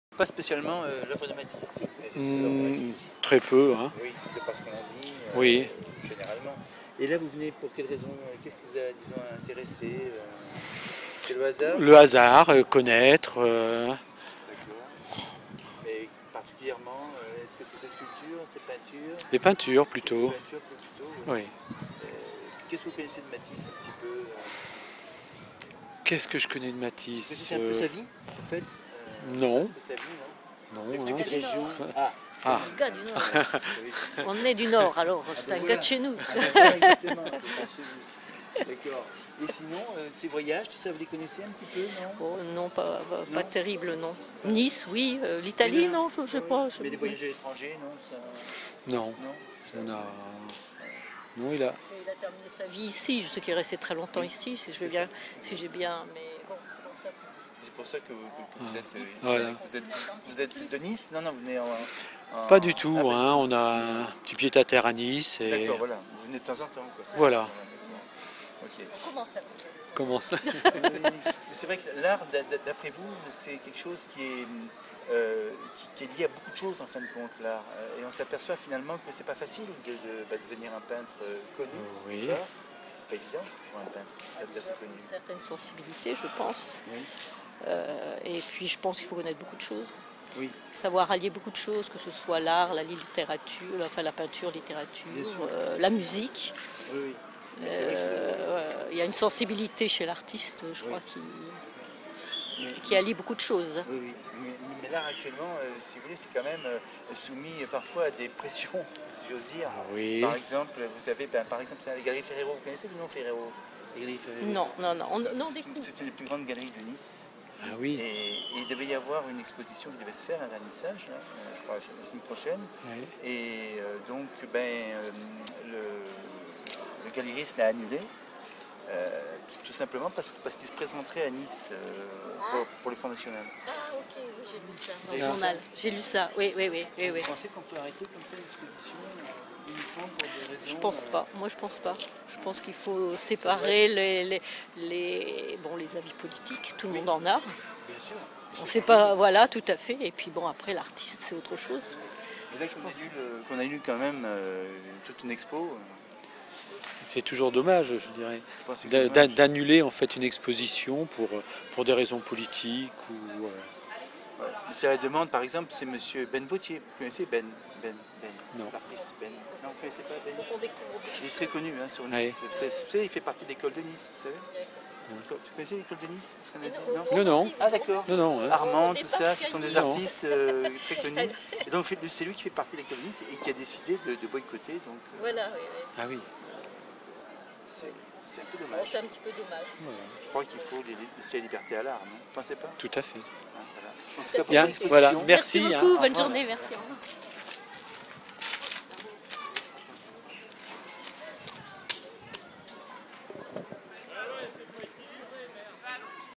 PAS D AMALGAME ENTRE L 'ART ET LA POLITIQUE
Actuellement se déroule l exposition MATISSE À NICE et les visiteurs ne souhaitent pas que l on annule des vernissages pour des raisons politiques  !!(  Comme pour la galerie FERRERO récemment .)